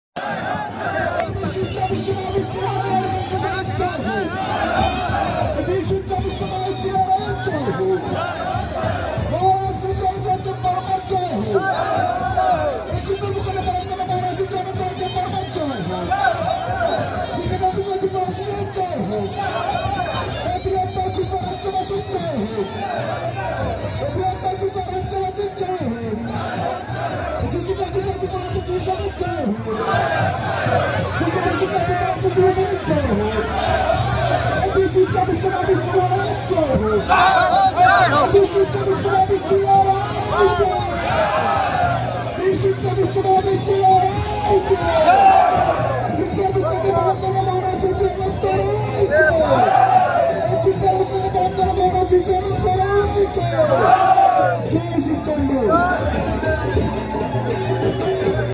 Vir Chila Ray 514th birthday anniversary at Kazakpara, Gosaigaon, Kokrajhar.(2)
Vir Chila Ray 514th  birthday anniversary at Kazakpara, Gosaigaon, Kokrajhar. @narayanbarmanofficial